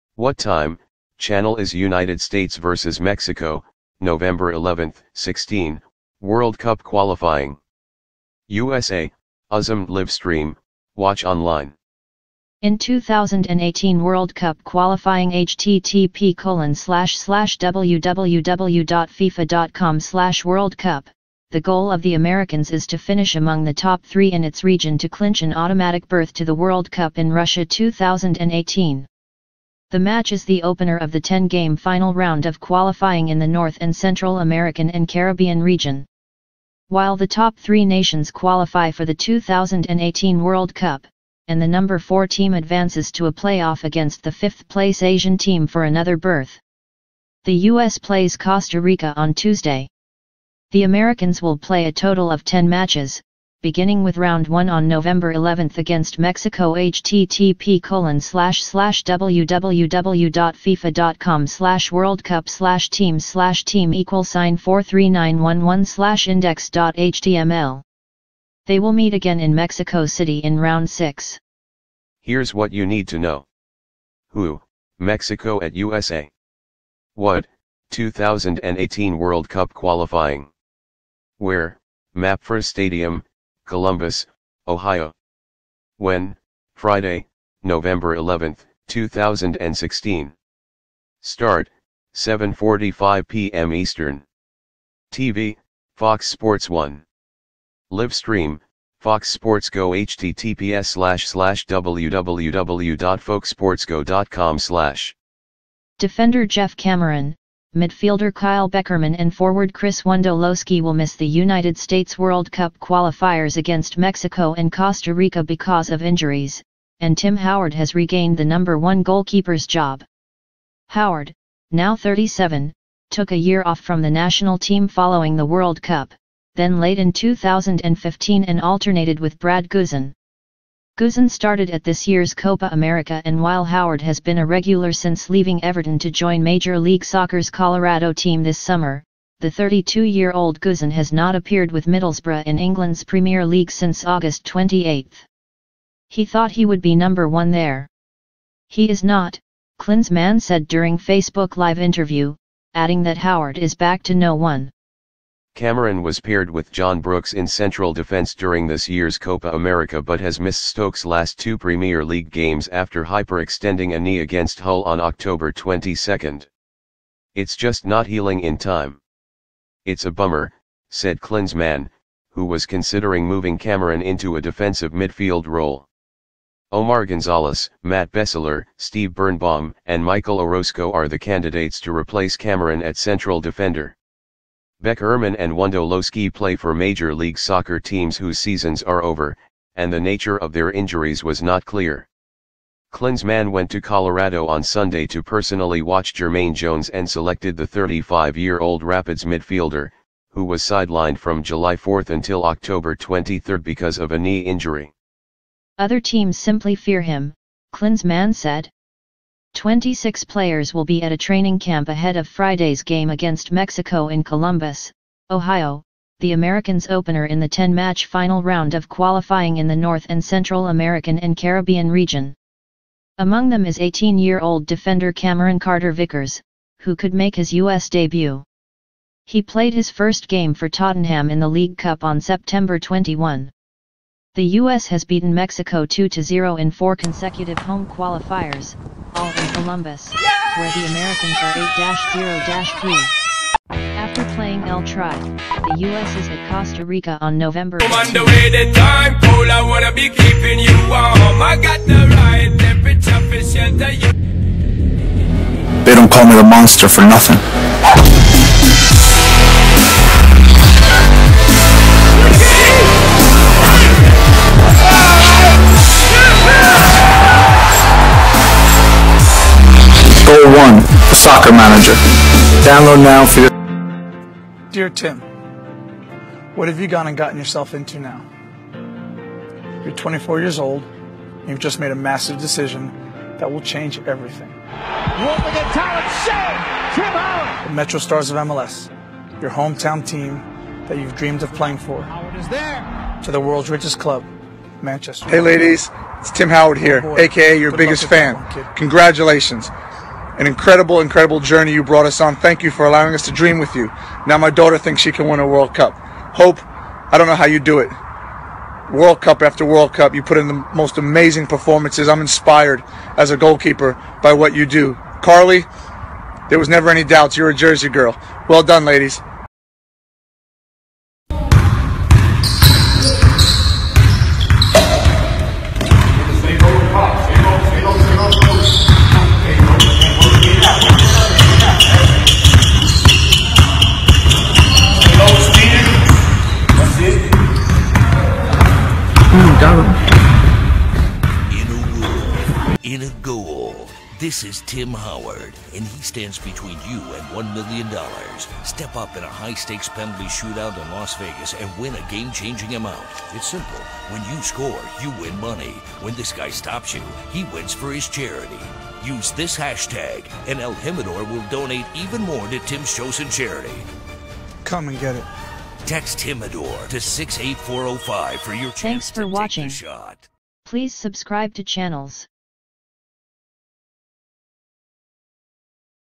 What time, channel is United States vs. Mexico World Cup qualifying? USMNT livestream, watch online
What time, channel is United States vs. Mexico (11/11/16) World Cup qualifying? USA, USMNT livestream, watch online
In 2018 World Cup qualifying, the goal of the Americans is to finish among the top three in its region to clinch an automatic berth to the World Cup in Russia 2018.
The match is the opener of the 10-game final round of qualifying in the North and Central American and Caribbean region. While the top three nations qualify for the 2018 World Cup, and the No. 4 team advances to a playoff against the fifth-place Asian team for another berth. The U.S. plays Costa Rica on Tuesday.
The Americans will play a total of 10 matches, beginning with Round 1 on Nov. 11 against Mexico. They will meet again in Mexico City in Round 6.
Here's what you need to know:
Who: Mexico at USA
What: 2018 World Cup qualifying
Where: MAPFRE Stadium, Columbus, Ohio
When: Friday, Nov. 11, 2016
Start: 7:45 p.m. Eastern
TV: Fox Sports 1
Livestream: Fox Sports Go
Defender Geoff Cameron, midfielder Kyle Beckerman and forward Chris Wondolowski will miss the United States' World Cup qualifiers against Mexico and Costa Rica because of injuries, and Tim Howard has regained the No. 1 goalkeeper's job.
Howard, now 37, took a year off from the national team following the World Cup, then late in 2015 and alternated with Brad Guzan.
Guzan started at this year's Copa America and while Howard has been a regular since leaving Everton to join Major League Soccer's Colorado team this summer, the 32-year-old Guzan has not appeared with Middlesbrough in England's Premier League since Aug. 28.
"He thought he would be No. 1 there. He is not," Klinsmann said during Facebook Live interview, adding that Howard "is back to No. 1."
Cameron was paired with John Brooks in central defense during this year's Copa America but has missed Stoke's last two Premier League games after hyperextending a knee against Hull on Oct. 22.
"It's just not healing in time. It's a bummer," said Klinsmann, who was considering moving Cameron into a defensive midfield role.
Omar Gonzalez, Matt Besler, Steve Birnbaum and Michael Orozco are the candidates to replace Cameron at central defender.
Beckerman and Wondolowski play for Major League Soccer teams whose seasons are over, and the nature of their injuries was not clear. Klinsmann went to Colorado on Sunday to personally watch Jermaine Jones and selected the 35-year-old Rapids midfielder, who was sidelined from July 4 until Oct. 23 because of a knee injury.
"Other teams simply fear him," Klinsmann said.
Twenty-six players will be at a training camp ahead of Friday's game against Mexico in Columbus, Ohio, the Americans' opener in the 10-match final round of qualifying in the North and Central American and Caribbean region. Among them is 18-year-old defender Cameron-Carter Vickers, who could make his U.S. debut. He played his first game for Tottenham in the League Cup on Sept. 21.
The U.S. has beaten Mexico 2-0 in four consecutive home qualifiers, all in Columbus, where the Americans are 8-0-3. After playing El Tri, the U.S. is at Costa Rica on Nov. 15.